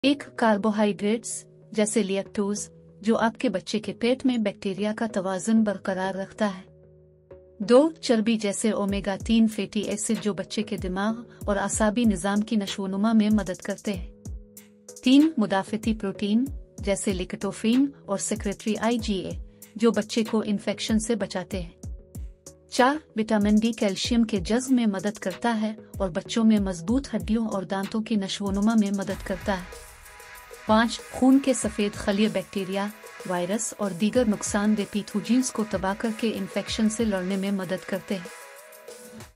چھاتی کے دودھ کی ساخت
کاربوہائیڈریٹس# اومیگا3_فیٹی_ایسڈ# مدافعتی_پروٹین# وٹامن_ڈی# سفید_خلیے

1۔ کاربوہائیڈریٹس، جیسے لییکٹوز، جو آپ کے بچے کے پیٹ میں بیکٹیریا کا توازن برقرار رکھتا ہے۔
 2. چربی جیسے اومیگا 3 فیٹی ایسڈ جو بچے کے دماغ اور اعصابی نظام کی نشوونما میں مدد کرتے ہیں۔
 3. مدافعتی پروٹین، جیسے لیکٹوفرین اور سیکریٹری آئی جی اے، جو بچے کو انفیکشن سے بچاتے ہیں۔
 4. وٹامن ڈی کیلشیم کے جذب میں مدد کرتا ہے اور بچوں میں مضبوط ہڈیوں اور دانتوں کی نشوونما میں مدد کرتا ہے۔
 5. خون کے سفید خلیے بیکٹیریا، وائرس اور دیگر نقصان دہ پیتھوجینز کو تباہ کرکے انفیکشن سے لڑنے میں مدد کرتے ہیں۔